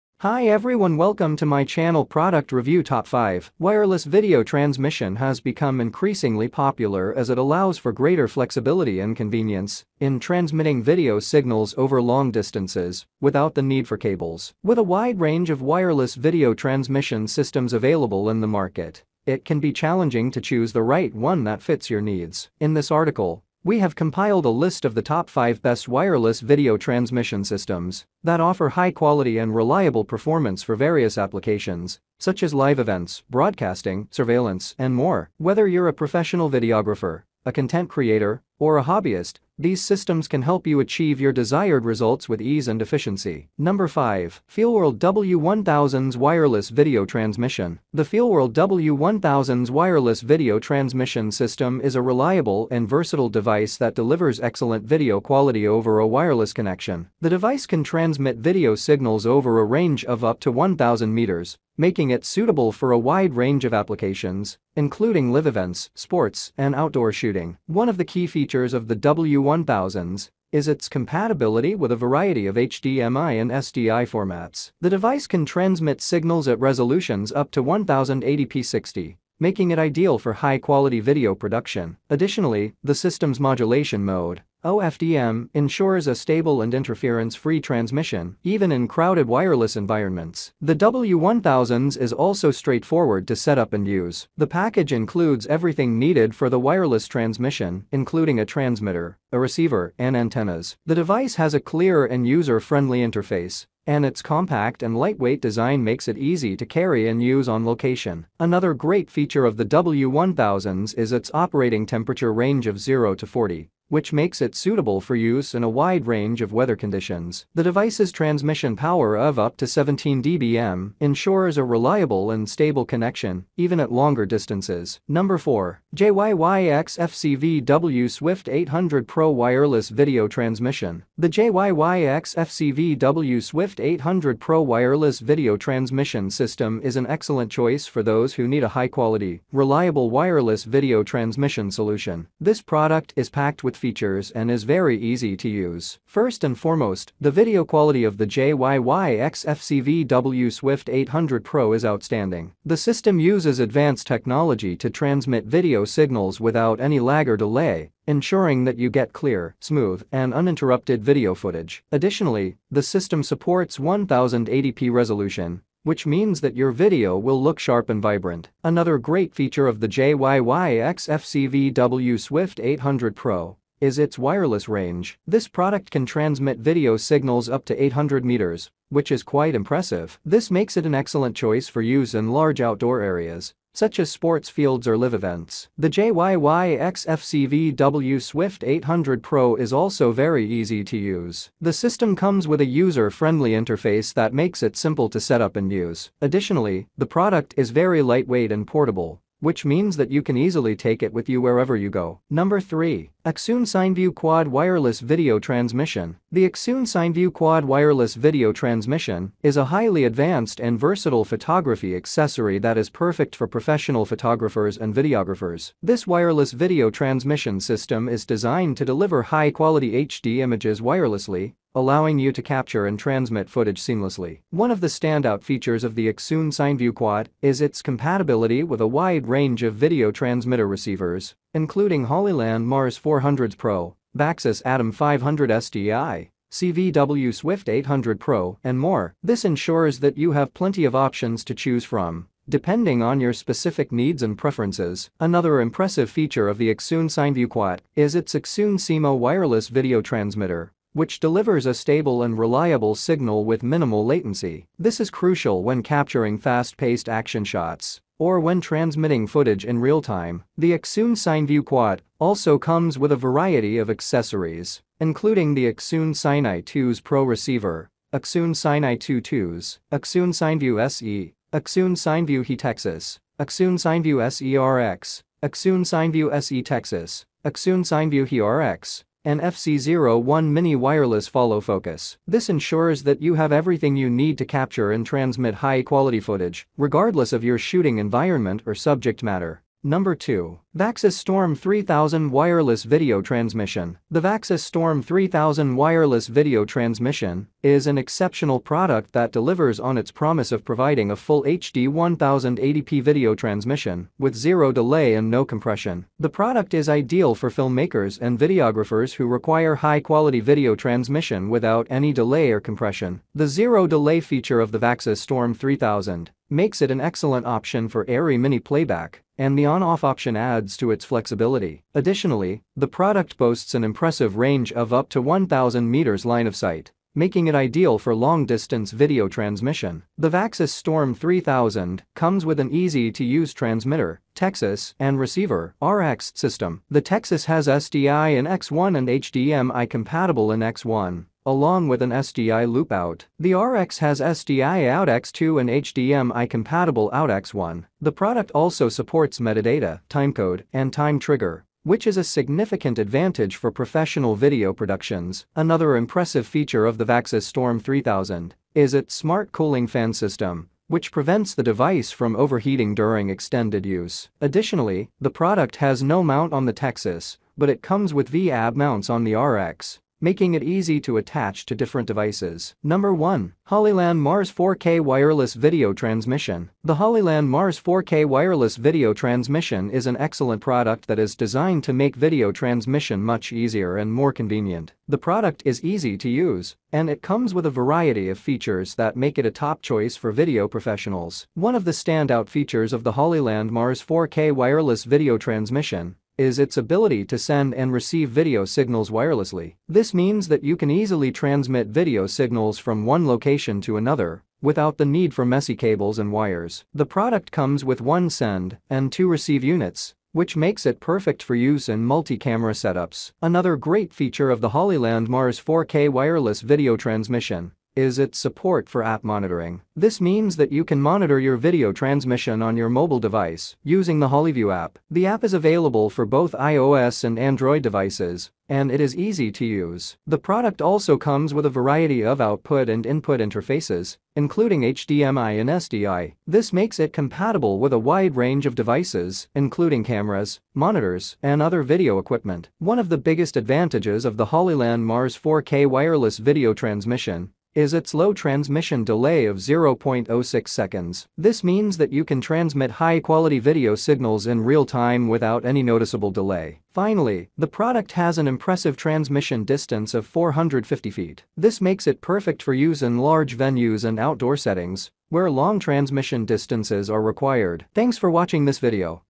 Top 5 Best Wireless Video Transmission 2023
Top 5 Best Wireless Video Transmission 2023. In this video, we review the Top 5 Best Wireless Video Transmission available in the market. Here are the top 5 best Wireless Video Transmission.

100 Best Wireless Video Transmission.

► 5. FEELWORLD W1000S Wireless Video Transmission

► 4. JYYXF CVW SWIFT 800pro Wireless Video Transmission

► 3. ACCSOON CineView QUAD Wireless Video Transmission

► 2. VAXIS Storm 3000 Wireless Video Transmission

► 1. HOLLYLAND Mars 4K Wireless Video Transmission

⏱️TIMESTAMPS⏱️
0:00 Intro
0:43 Number 5: FEELWORLD W1000S Wireless Video Transmission
2:04 Number 4: JYYXF CVW SWIFT 800pro Wireless Video Transmission
3:22 Number 3: ACCSOON CineView QUAD Wireless Video Transmission
4:49 Number 2: VAXIS Storm 3000 Wireless Video Transmission
6:12 Number 1: HOLLYLAND Mars 4K Wireless Video Transmission